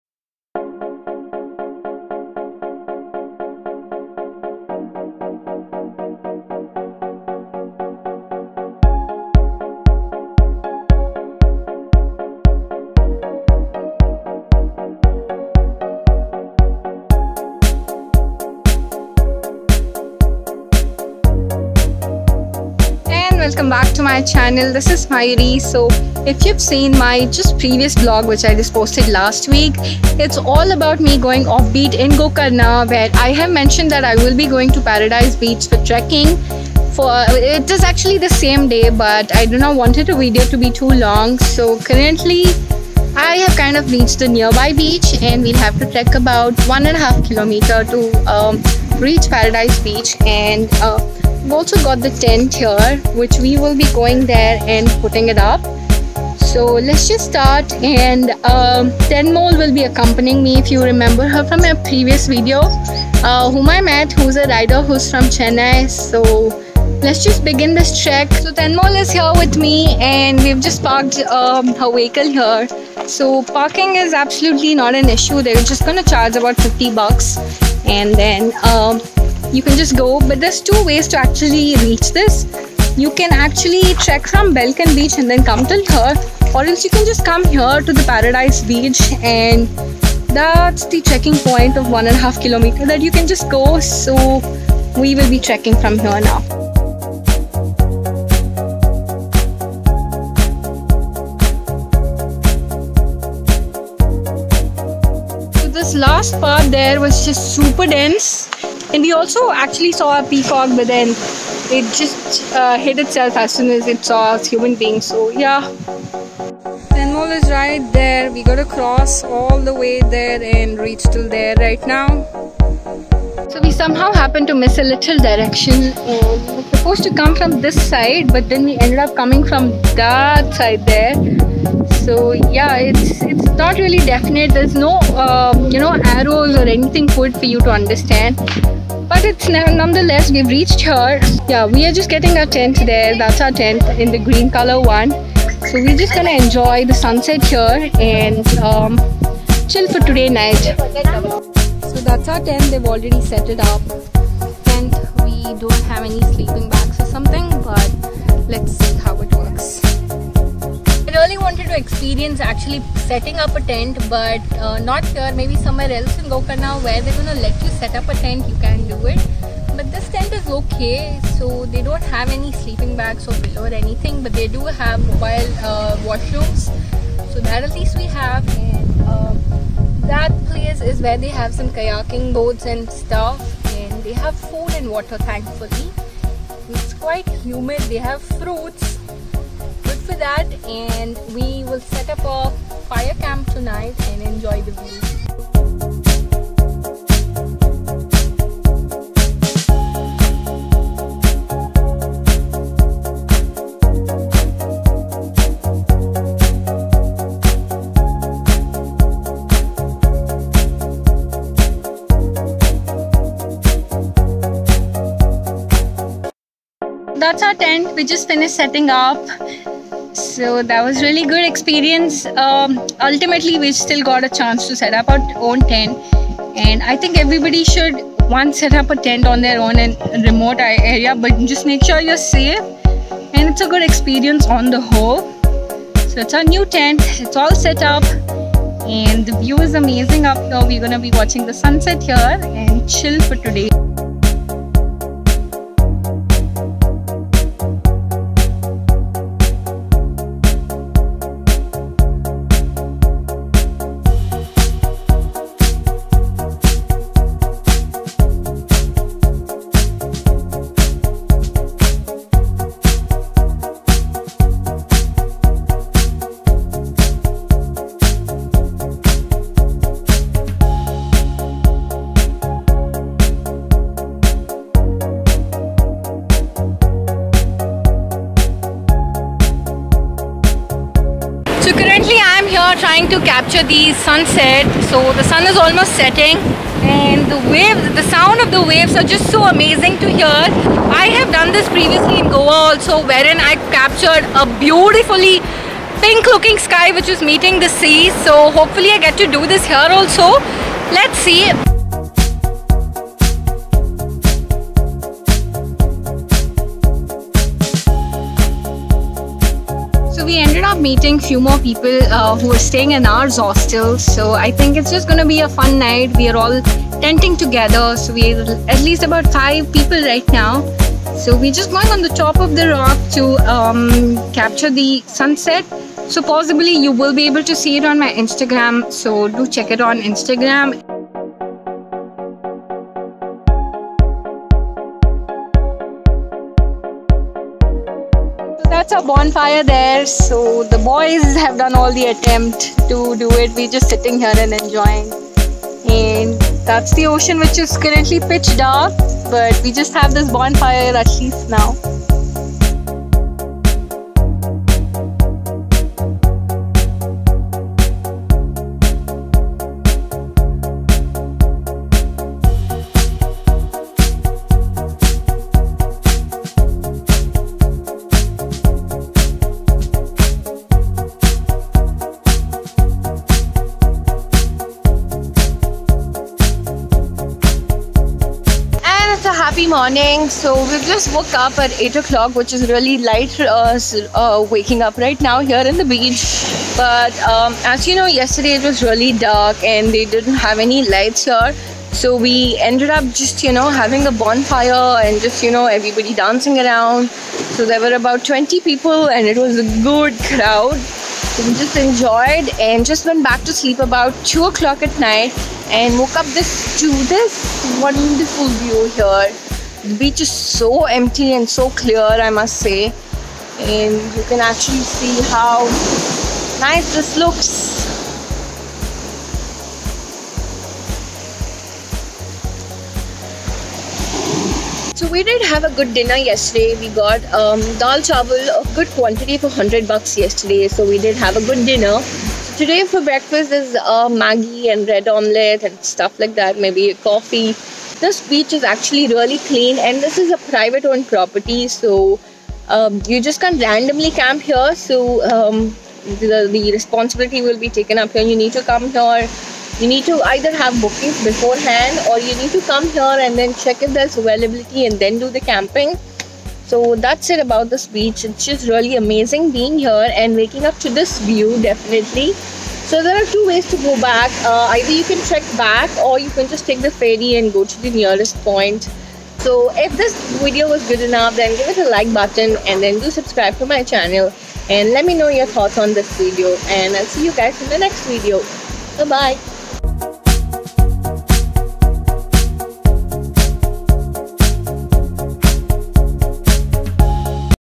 Paradise Beach Gokarna | Hidden private Beach | Night camp | Vlog 2
Paradise beach's name in itself says that it provides an experience such as a paradise. This is a privately owned property and is open for people camping overnight. There are very minimal resources available and is most likely for people who want to connect with Nature.